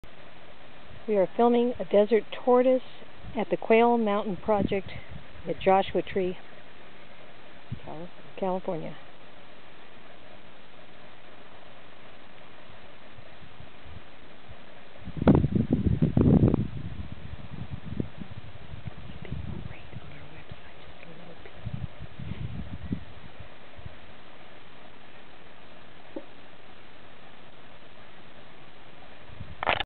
Mojave Land Trust - Joshua Tree Tortoise - Quail Mountain Project
Tortoise in Joshua Tree - potential acquisition for Mojave Land Trust - Quail Mountain Project. Tortoise eating dandelions.